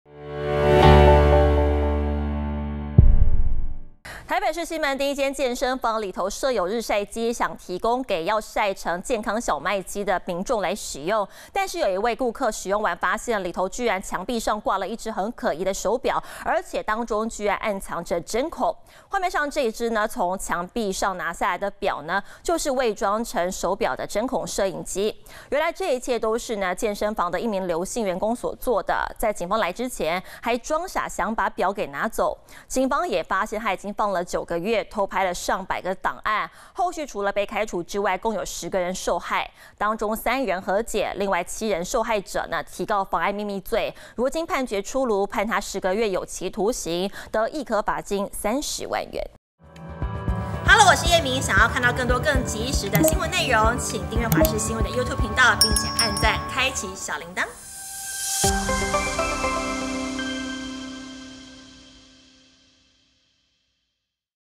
日曬房藏針孔! 健身房男員工判10月徒刑｜華視新聞 20220326
台北市西門町一間件健身房，裡面設有日曬機，提供想曬成健康小麥肌的民眾使用，但是有1位顧客使用完之後，竟然發現牆壁上掛著一支可疑的手錶，裡面竟然暗藏著針孔。